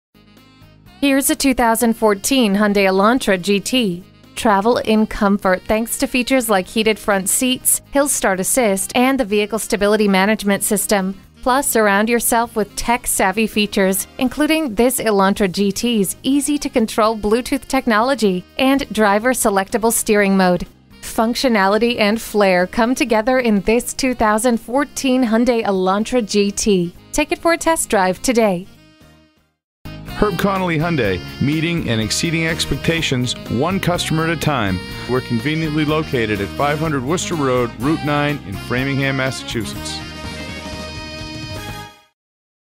2014 Hyundai Elantra GT Framingham Boston, MA #13060 - SOLD
Year: 2014
Make: Hyundai
Model: Elantra GT
Engine: 2.0L
Transmission: 6-Speed Automatic with Overdrive
Color: Black
Interior: Black
Stock #: 13060
VIN: KMHD35LH8EU215385
ABS brakes, Alloy wheels, Bluetooth Hands-Free Phone System, Cargo Net, Cargo Tray, Carpeted Floor Mats, Electronic Stability Control, Heated door mirrors, Heated front seats, Illuminated entry, I-Pod and USB Inputs, Low tire pressure warning, Power windows, Remote keyless entry, Satellite Radio and Traction control. Price includes: $1,500 - Retail Bonus Cash. Exp. Exp. 09/02

Address:
500 Worcester Rd - Rte 9 East